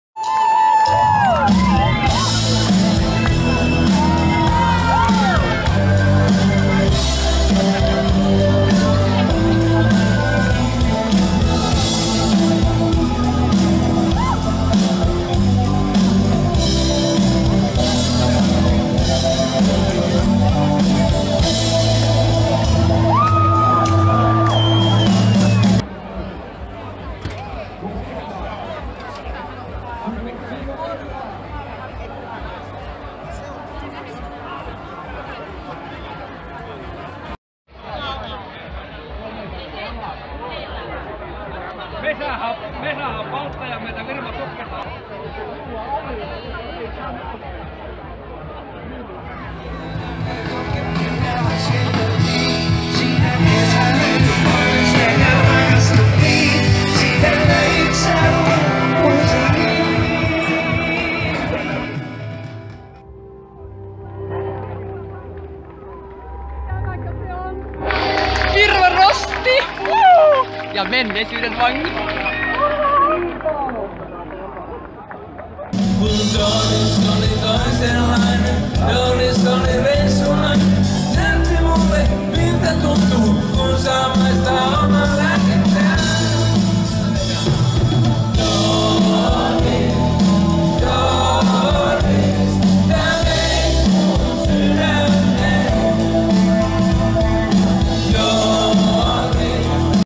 @dorothea2017
Beautiful Dooris attending musicfestival in Finland. She has been living in America for years, and her arrival makes a huge difference, she is almost a superstar..at least, that what she thinks.. Part of 100Stories Summer challenge , Finland 2017.